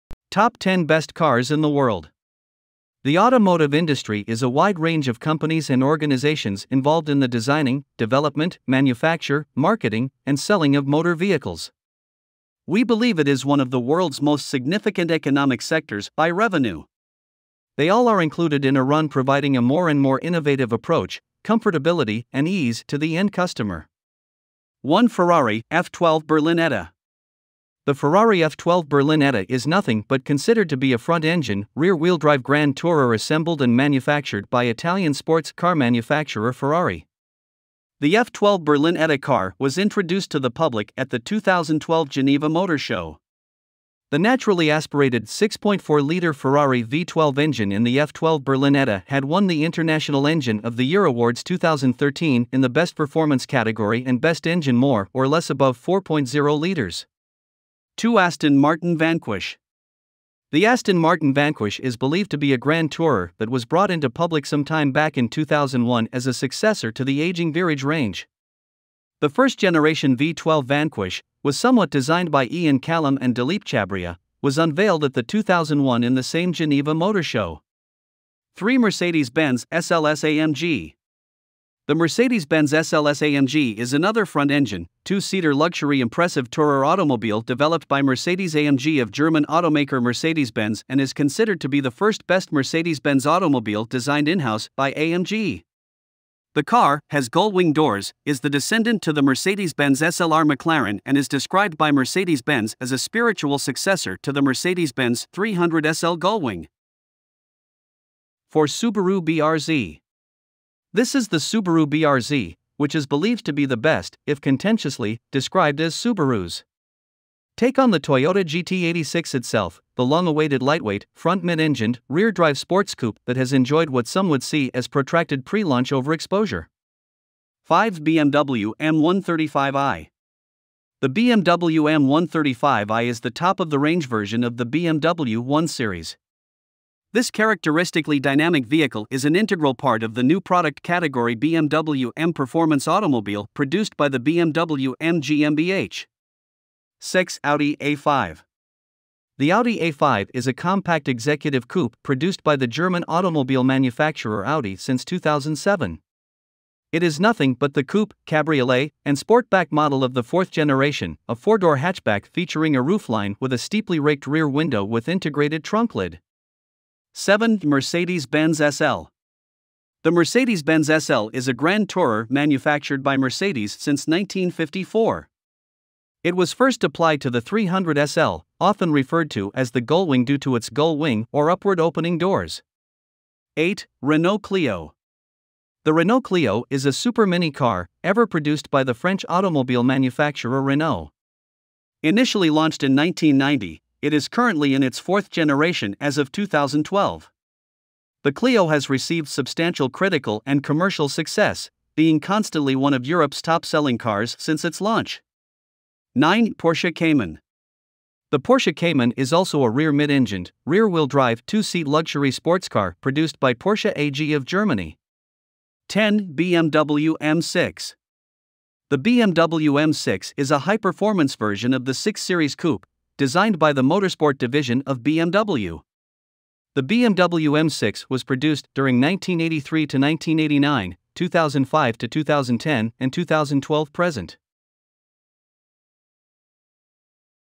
Top 10 best cars in the world | Review | The tech future
In this video, we're taking a look at the Top 10 best cars in the World! Super machines that can walk and even climb, to sleek and futuristic machines that can fly, this list is sure to impress!

When it comes to the world's fastest cars, the speeds achieved are insane.

Here is the list:
1:- Ferrari F12 Berlinetta
2:- Aston Martin Vanquish
3:- Mercedes Bens SLS AMG
4:- Subaru BRZ
5:-BMW M135i
6:-Audi A5
7:-Mercedes Bens SL
8:-Renault Clio
9:-Porsche Cayman
10:-BMW M6